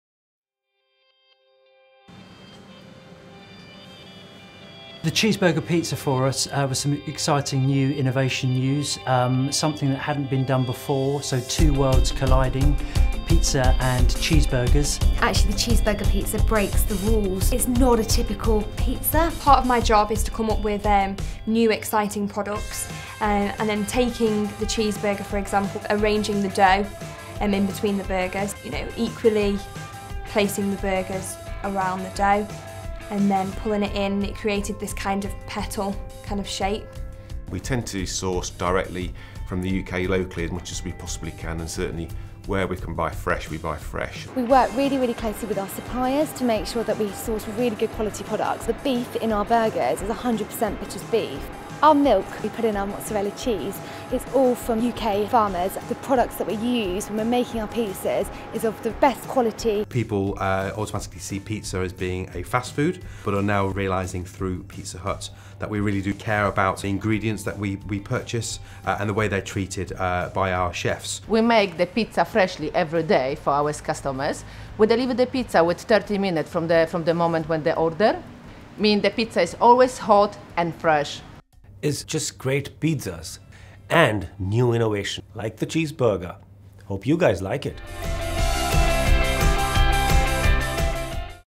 Pizza Hut Innovates with the Cheeseburger Pizza
Pizza Hut UK have created the Cheeseburger Pizza. Check out this video!